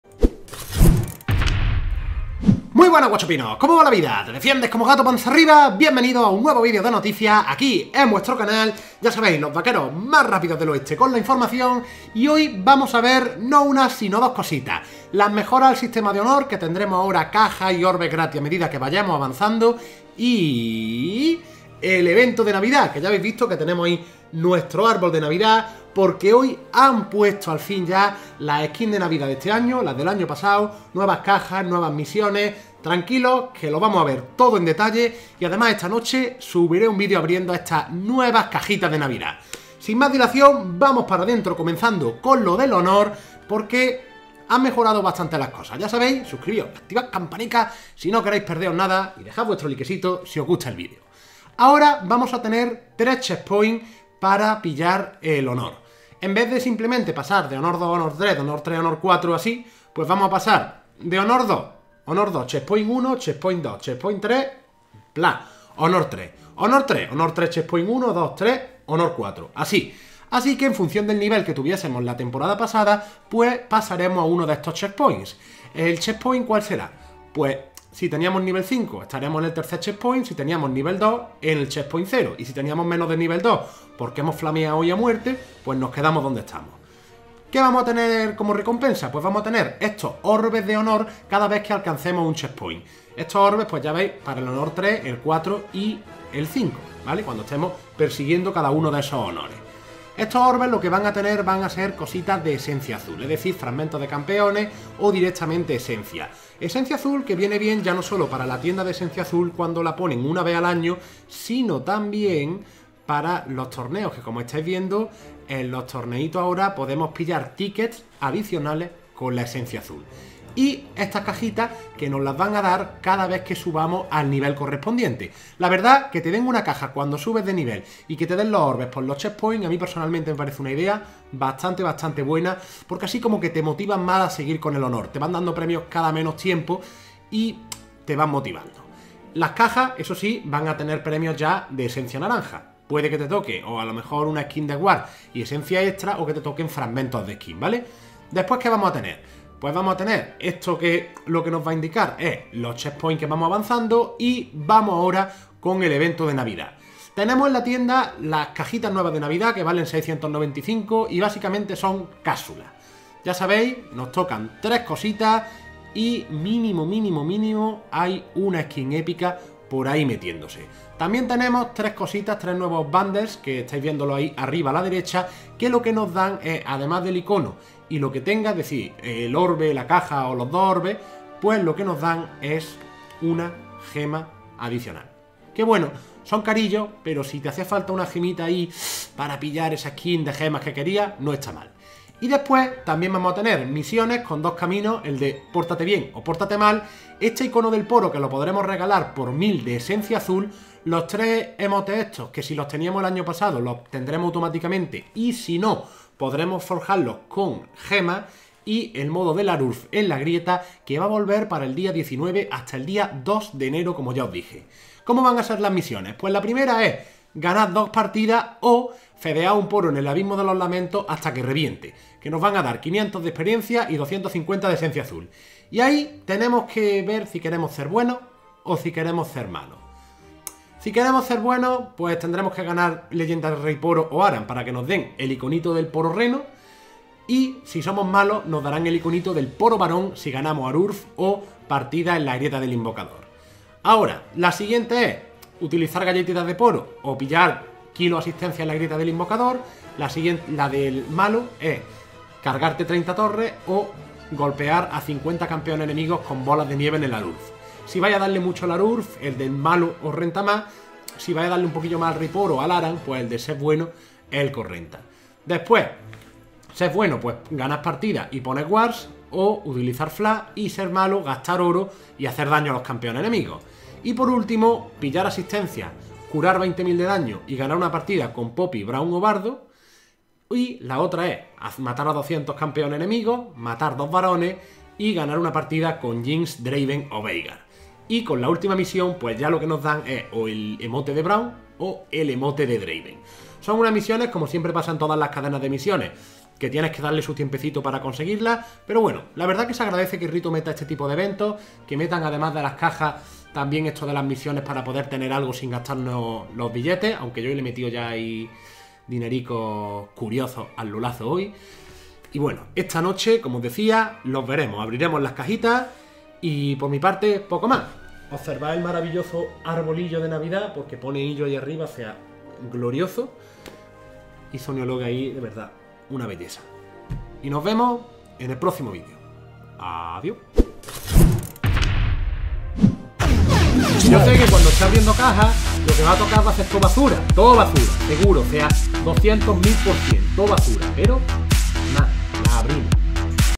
NUEVAS CAJAS y MISIONES - Evento NAVIDAD y CAMBIOS HONOR | Noticias League Of Legends LoL
¡Llega la navidad a League Of Legends!¡Nuevas cajas hextech y misiones! Además, cambios al honor para tener más checkpoints y más cositas.

MÜSICA: Playlist stream music copyleft.

Mención especial a TheFatRat, Tobu, Rakhous y el último álbum de música de Riot. Si conoces más canciones buenas sin copyright, pásamelas y las metemos en la lista ^^